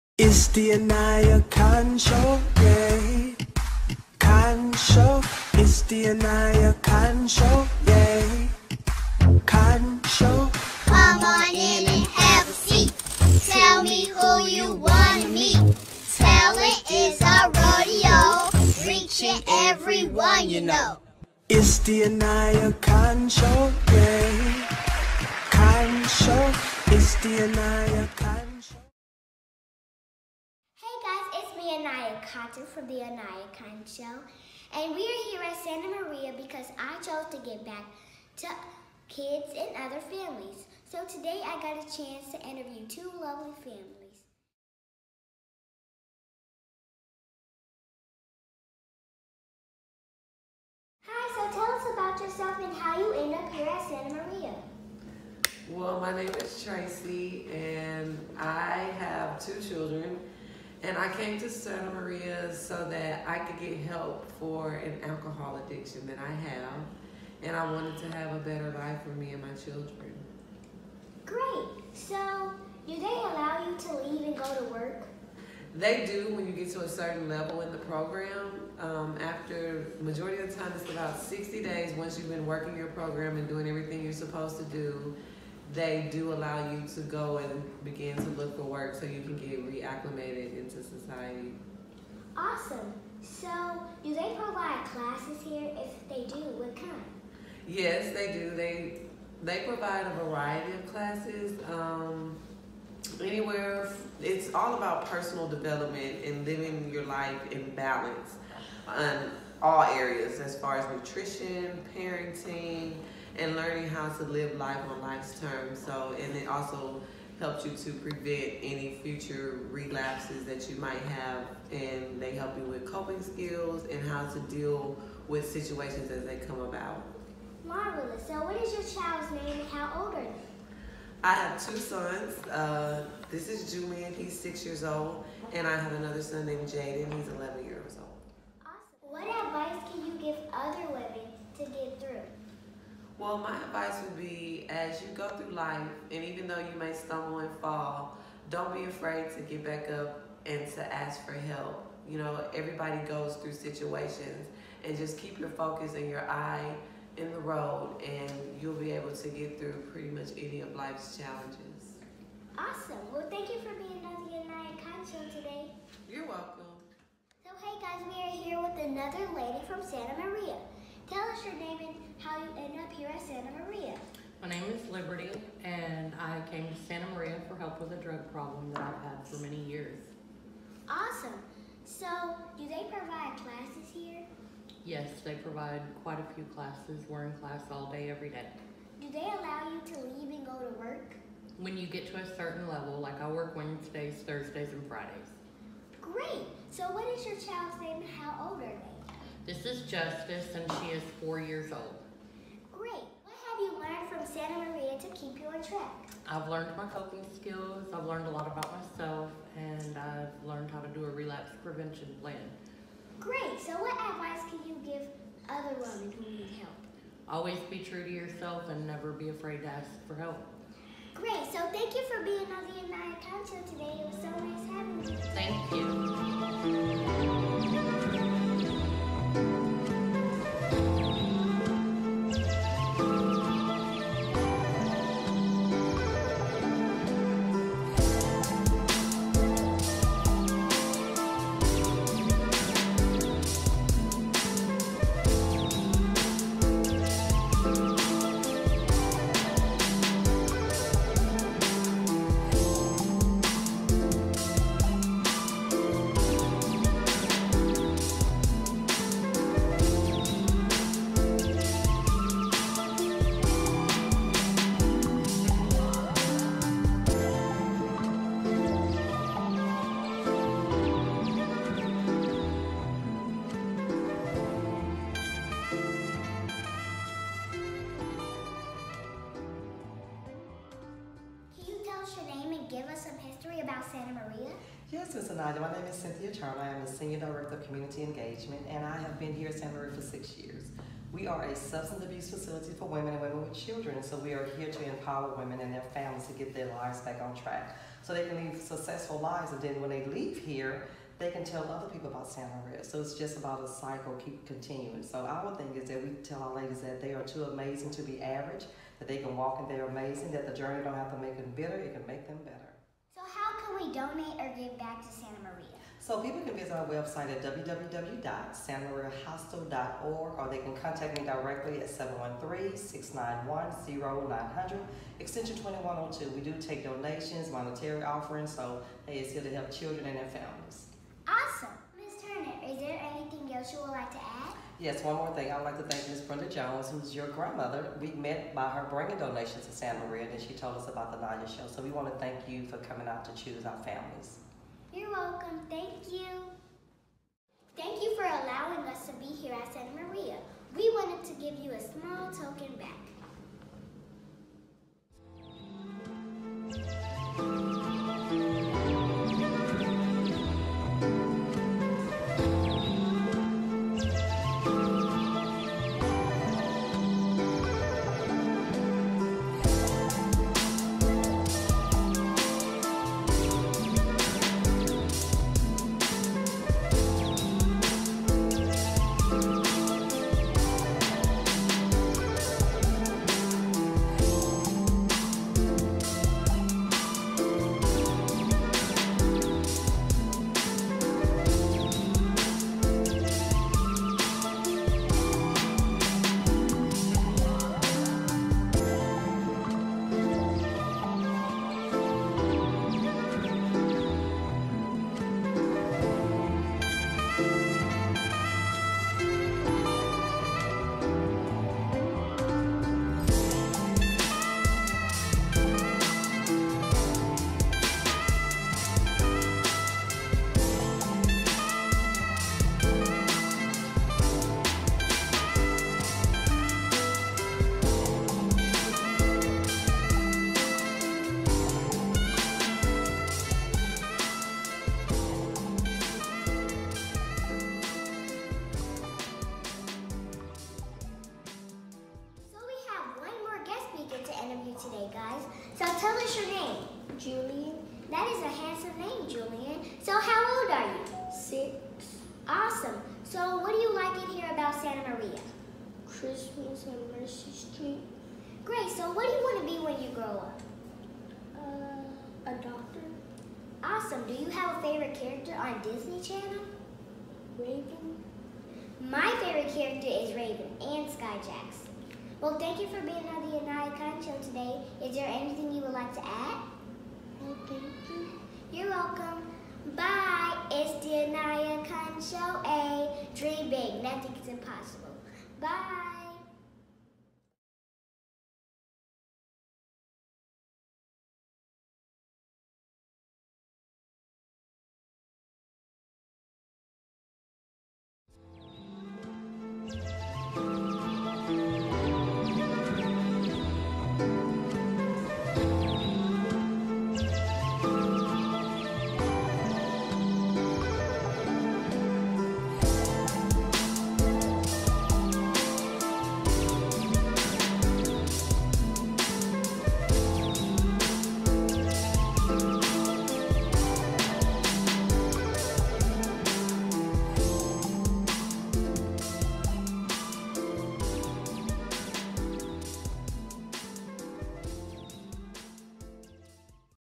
Aniyah at Santa Maria Hostel
Santa Maria Hostel is a warm and welcoming home for woman and there kids who need help in recovery